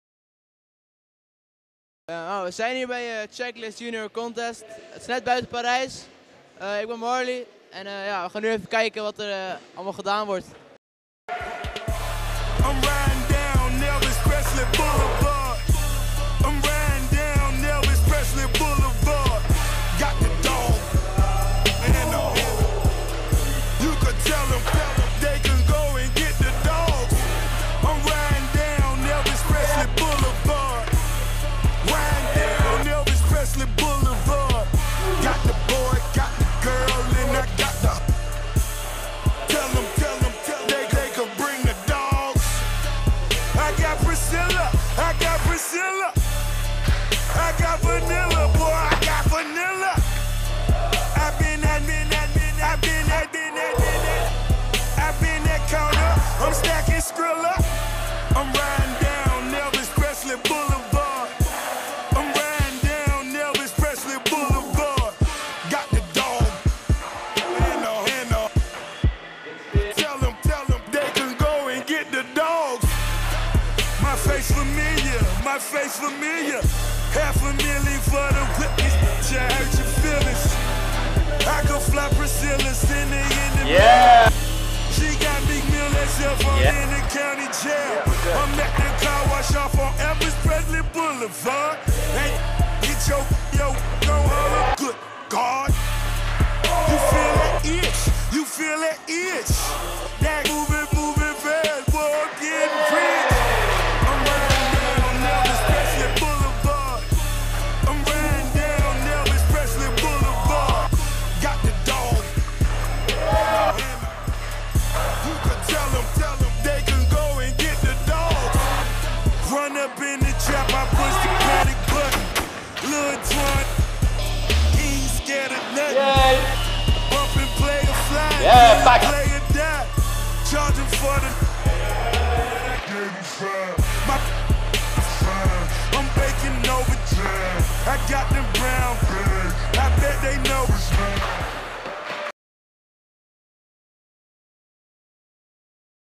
Checklist Junior Contest Parijs (2015)
Zaterdag 24 januari stond het skatepark in Villiers-sur-Orge in het teken van de Checklist Junior Contest. Check hier onze video van deze dag!